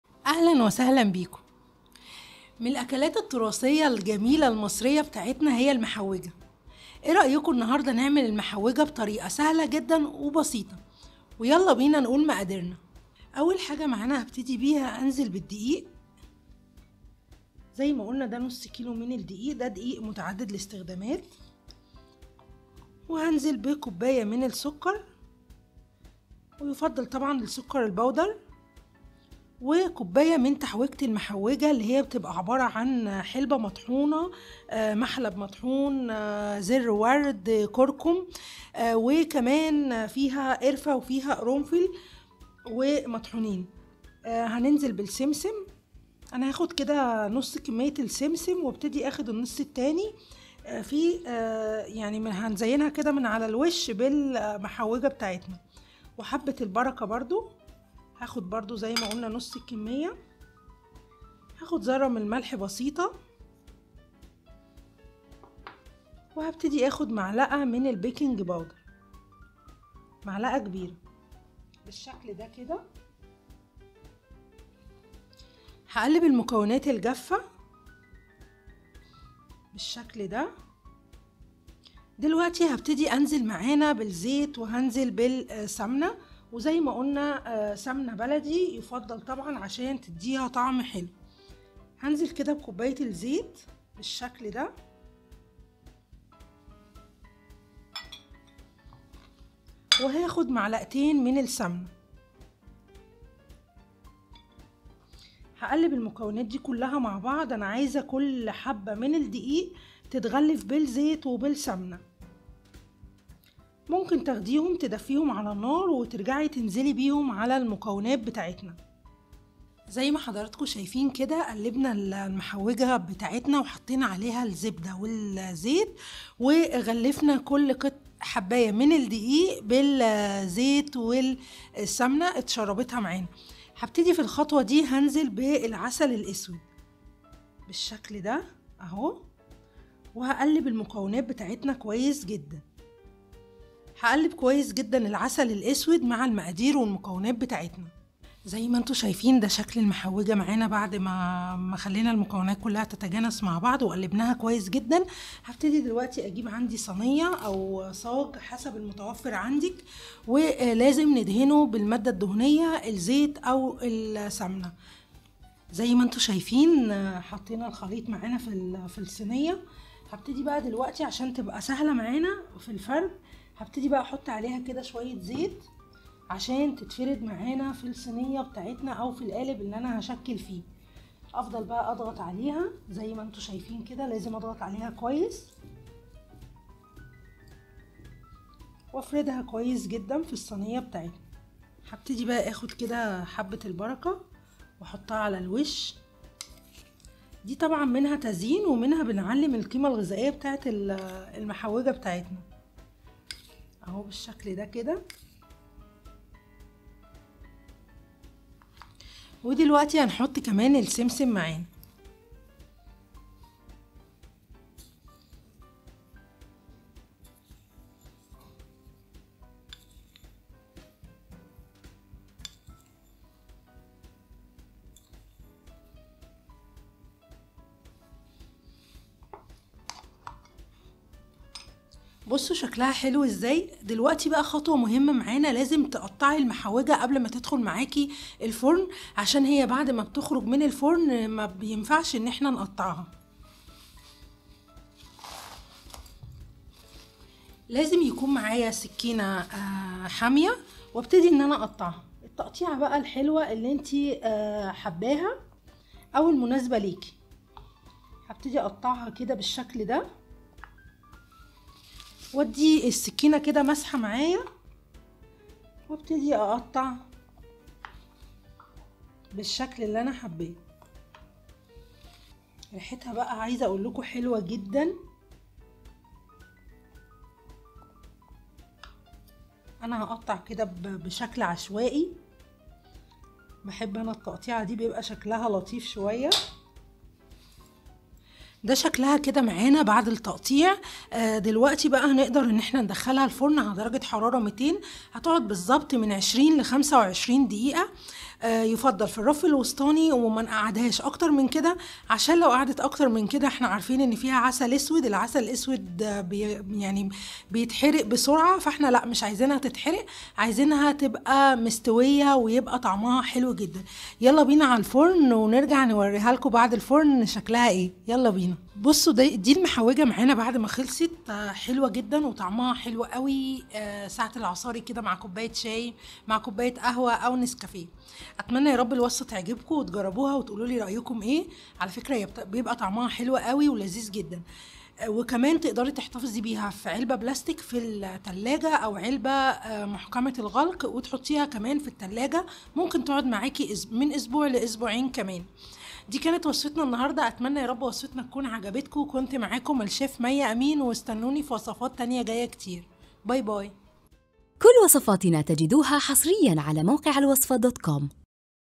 من اشهر الاكلات المصرية قديما "المحوجة " اعرفي الطريقة المصرية الاصلية لعمل قرص المحوجة
طريقة عمل المحوجة المصرية على اصولها
المقادير
1\2 كيلو دقيق
1 كوب سكر بودر
1 كوب تتبيلة المحوجة
8 معلقة سمسم
4 معلقة حبة البركة
رشة ملح
1 معلقة كبيرة بيكينج بودر
1 كوب زيت
2 معلقة كبيرة سمن
1\2 كوب عسا اسود